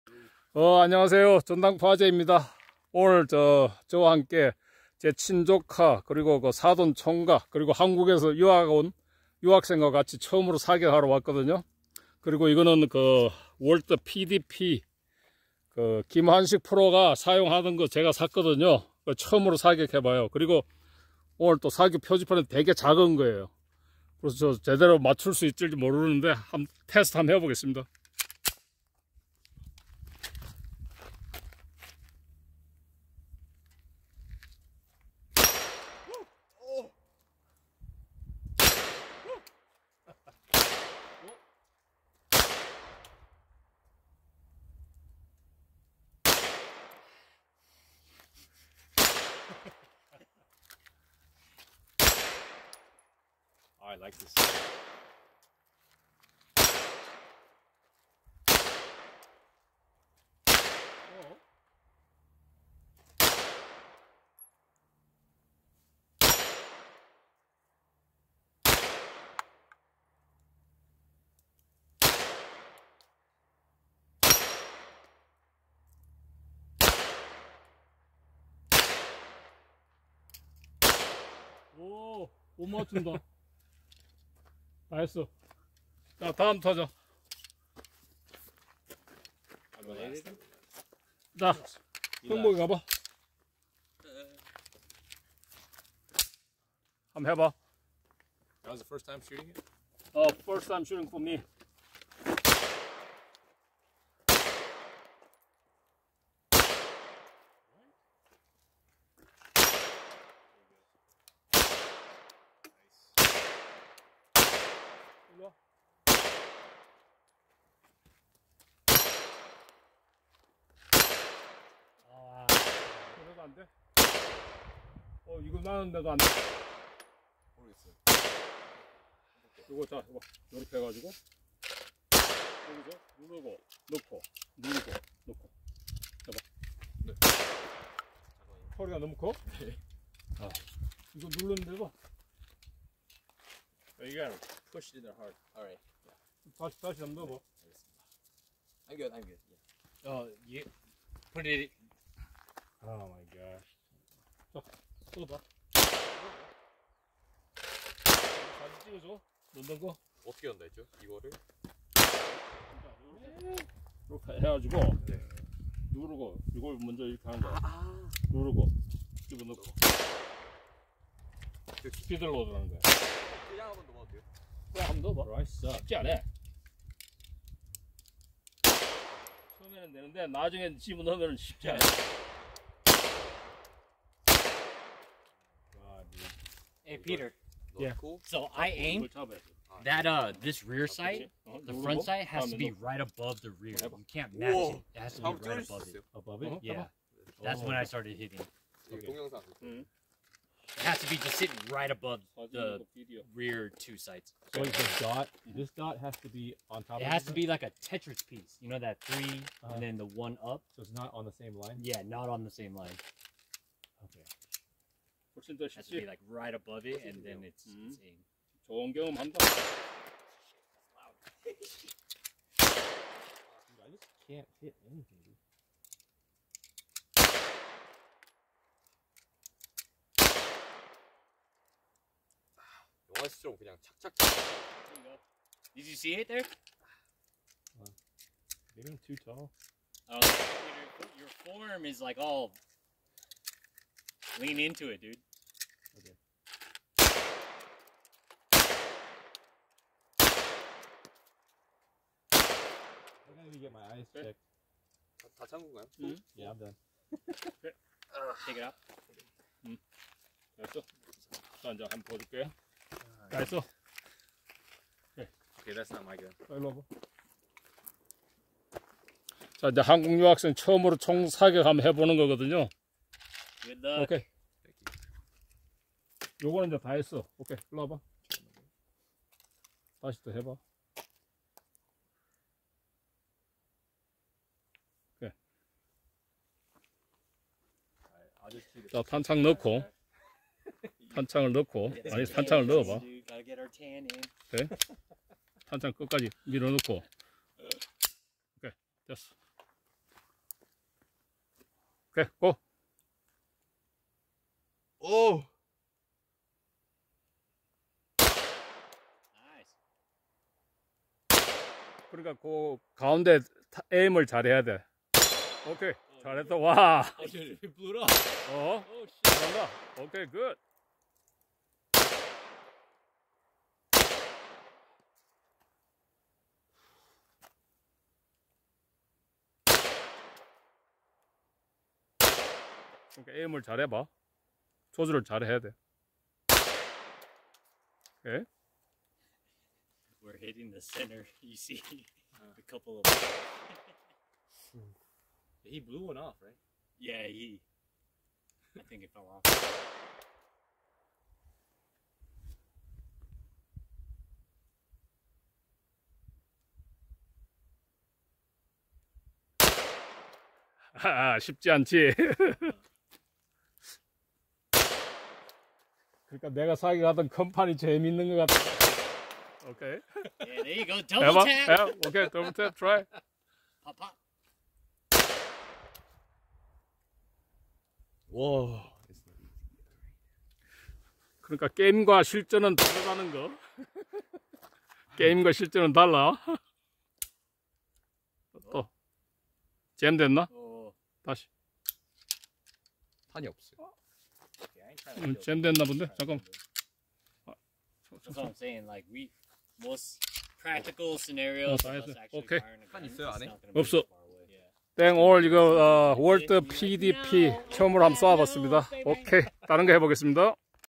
야산 사격 : 올해 들어서 첨으로 야산에서 Walther PDP 9mm 반자동 권총을 사격 하는 영상입니다.
Walther PDP 9mm 반자동 권총은 미국 프로 사격선수 김환식 프로가 사용하던 권총을 제가 착한 가격에 구매해서 사격하러 갔습니다. 제가 실력이 없어서 Walther PDP 9mm 반자동 권총은 좋지만 사격 표지판이 작아서 제대로 맞추지 못했습니다. ㅜㅜ